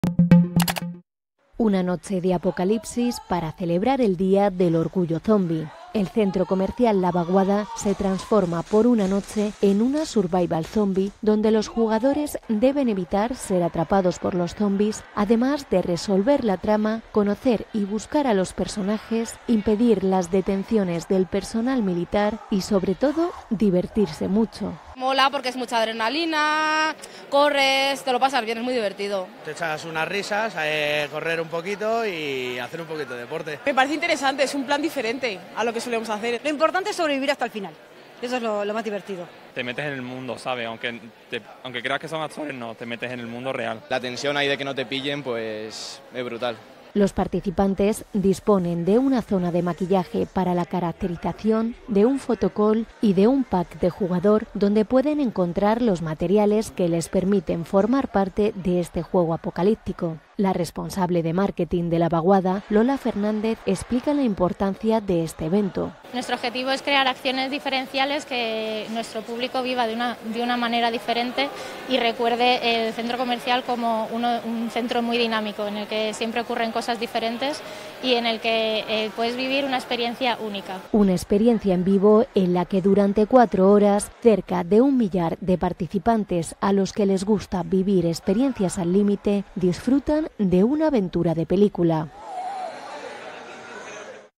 Una noche de apocalipsis para celebrar el curioso “Día del Orgullo Zombi”
El centro comercial La Vaguada se transforma, por una noche, en una 'Survival Zombie', una zona de juego de supervivencia para todos aquellos a los que les gusta vivir experiencias al límite sumergiéndose en un escenario post apocalíptico.

¡Visítenos!